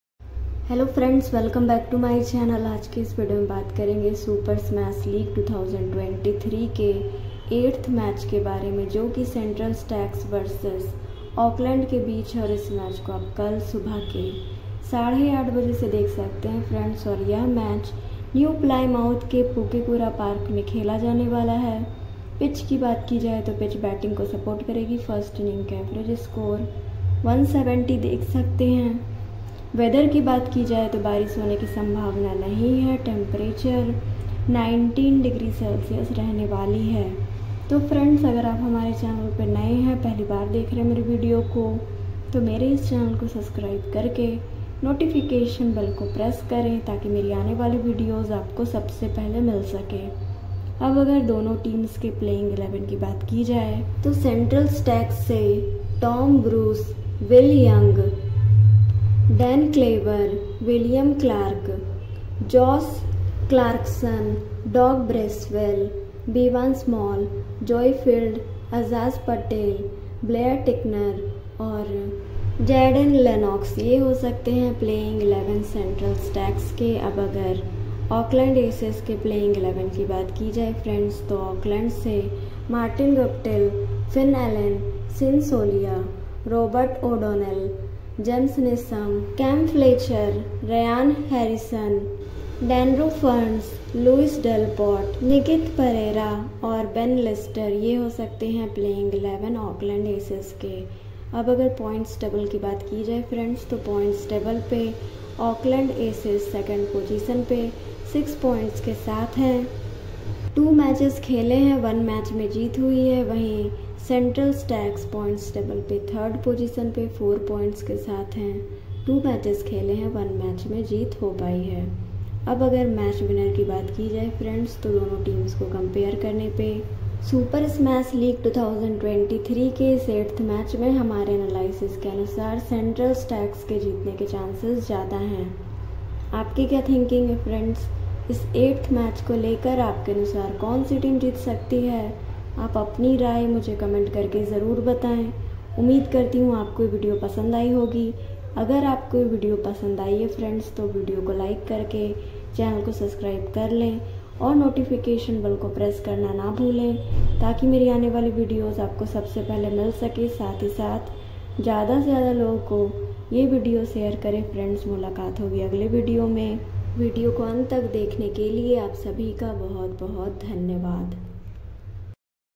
Super smash league 2023 Central stags vs Auckland Aces 8th Match prediction today cs vs auck
Central Stags will be up against the Auckland Aces in the 8th match of the Super Smash 2023. The match will be played at Pukekura Park, New Plymouth and will start at 8:30 AM IST on December 29.

Central Stags have played two matches in the Super Smash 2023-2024 so far, both against Northern Knights. They registered a massive 55 run defeat in the first game but bounced back to bag a 56 run victory in the next game.

Meanwhile, Auckland Aces won the first game against Canterbury by 28 runs before their second game against Wellington was washed out due to rain, It is very competitive tournament thus both teams would like to take a lead early in the tournament. A defeat would not be good for either of the team.

Central Stags vs Auckland Aces T20 Match Live Streaming

TV channels: Star Sports has the broadcast rights of Super Smash and fans can watch the matches on Star Sports Network.

Live Streaming: You can live-stream the match on Hotstar and Website.

CS vs AA Match Toss Prediction

Chasing has been preferred by captains so far and once again captain winning the toss should bowl first.

CS vs AA Pitch Report

The pitch will help the bowlers early but it will settle down to become a very good batting surface. A score of 170 should be a good one to defend.